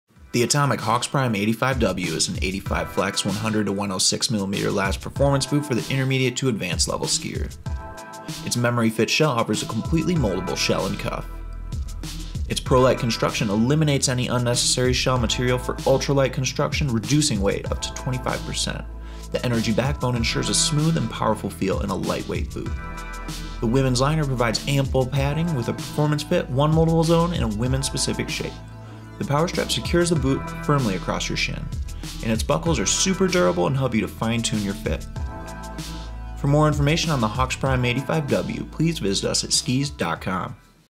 2019 Atomic Hawx Prime 85 W Women's Boot Overview by SkisDotCom
The Atomic Hawx Prime 85 W makes a great choice for the intermediate to advanced skier who has a medium width forefoot and medium size calf volume. Atomic's Memory Fit Shell allows an Authorized Atomic Dealer to heat the shell in a special oven. The bootfitter will then place the boot on your foot, as the shell cools it will start to mold and adapt to the shape of your foot. The Memory Fit 3D Silver Liner also has heat moldable zones in the heel and ankle pocket that add to the level of customization. The Women's Specific Cuff has an Adaptive Fit System Cuff that gives you the ability to fine tune the fit across your calf muscle for total comfort. Atomic's new Prolite Technology has changed the way they construct ski boots. First thin and lightweight materials are put into place. Then reinforcements are added only in strategic areas to improve performance. An Energy Backbone delivers a strong forward flex and added rebound to each turn. If you want performance, comfort and customization all wrapped up into a lightweight ski boot, the Atomic Hawx Prime 85 W should be tops on your list.